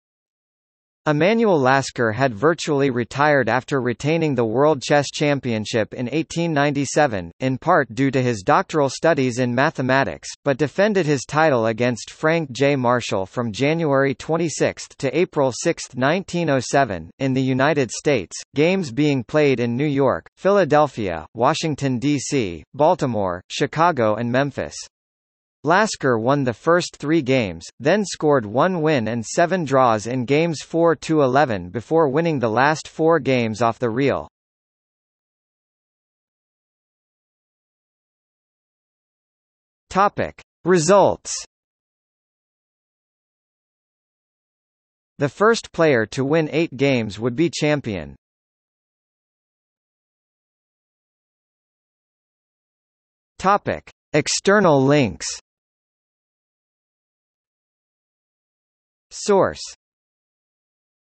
World Chess Championship 1907 | Wikipedia audio article
00:00:36 Results
00:00:49 External links

- Socrates

SUMMARY
Emanuel Lasker had virtually retired after retaining the World Chess Championship in 1897, in part due to his doctoral studies in mathematics, but defended his title against Frank J. Marshall from January 26 to April 6, 1907, in the United States, games being played in New York, Philadelphia, Washington, D.C., Baltimore, Chicago and Memphis. Lasker won the first three games, then scored one win and seven draws in games 4-11 before winning the last four games off the reel.